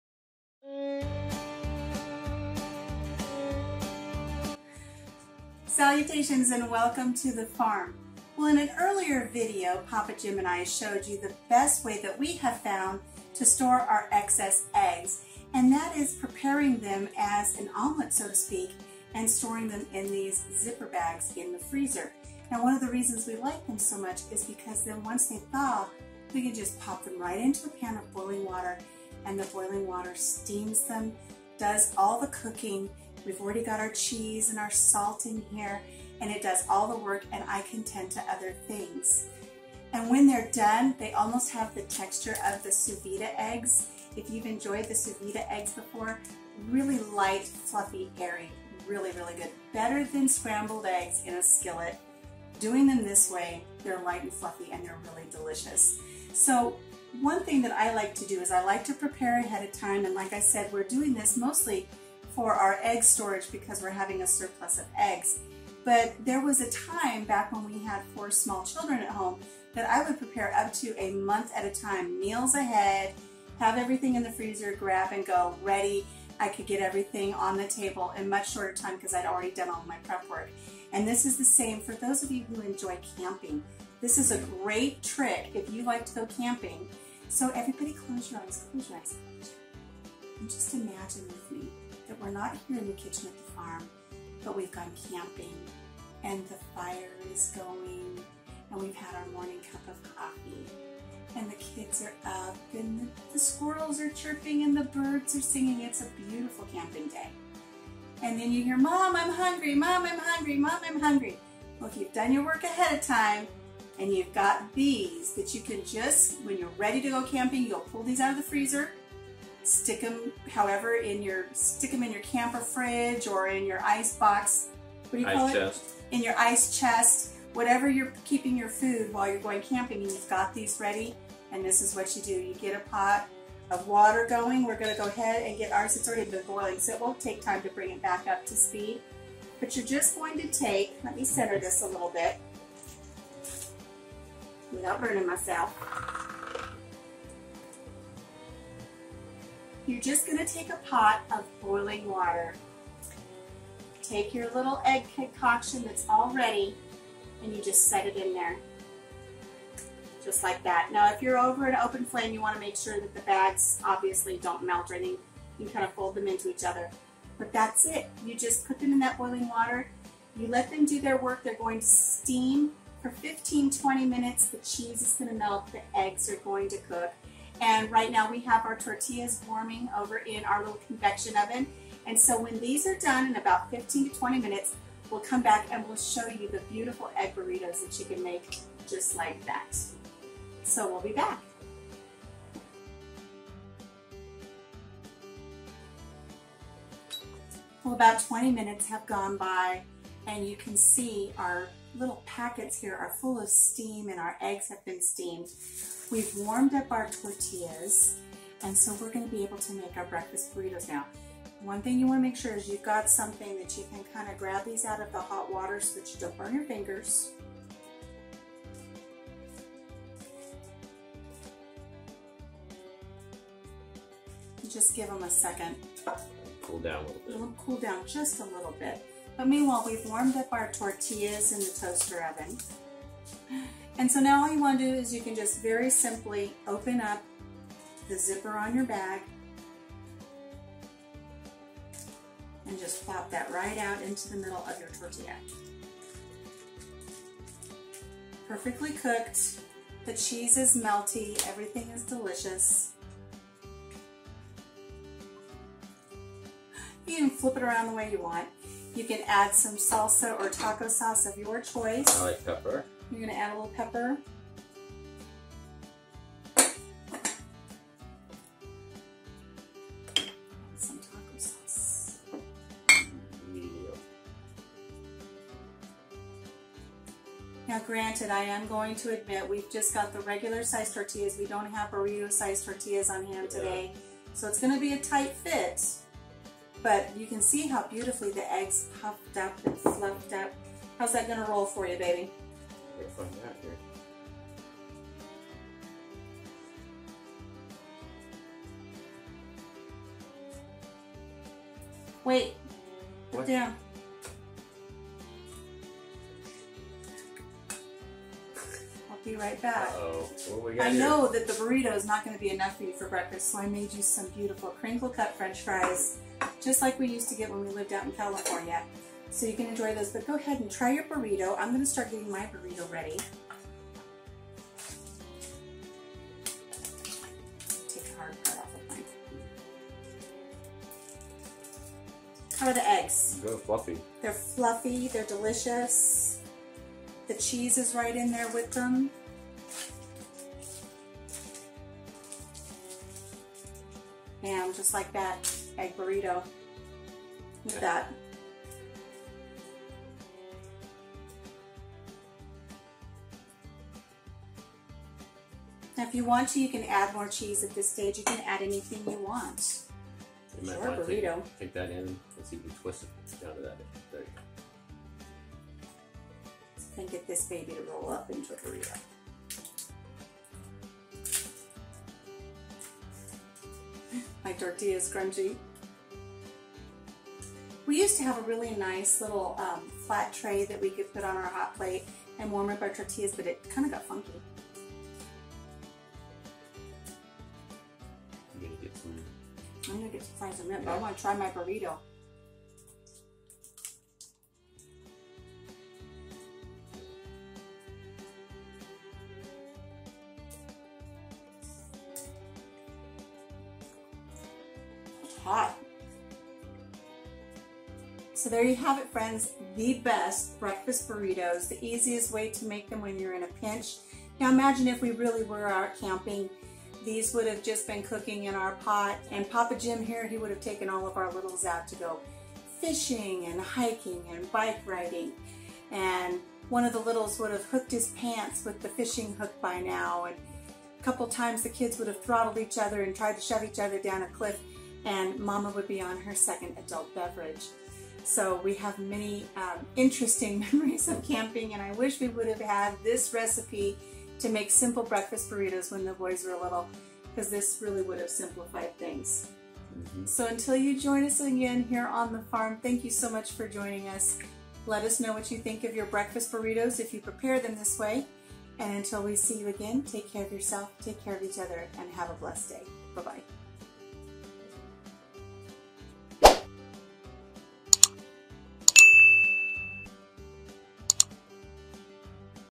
Breakfast Burritos ~ The Easy Way
On a beautiful, chilly morning, there's nothing we like better than a hearty, cheesy, tasty egg breakfast burrito!
And here is the absolute easiest way we've found to make them; steam the eggs in freezer bags!
So simple, so delicious... Perfect for camping. You can even put the egg packets together AT your campsite and let each camper add the ingredients of their choosing; sausage crumbles, bacon bits, ham chunks, peppers, onion, etc!
Now I confess, I mucked up the tortillas ( ordering a new tortilla pan as I upload this video) but, it didn't diminish the beauty of those fluffy, perfectly steamed eggs!
These steamed eggs, as shown in this video, also make up the perfect omelet! Just imagine it served up with a beautiful white gravy and shredded cheddar cheese on top! ...and maybe some country fried potatoes on the side ... Delish !!!
Be sure to let me know if you try these. They're so simple!

Art Credit: Duane Bryer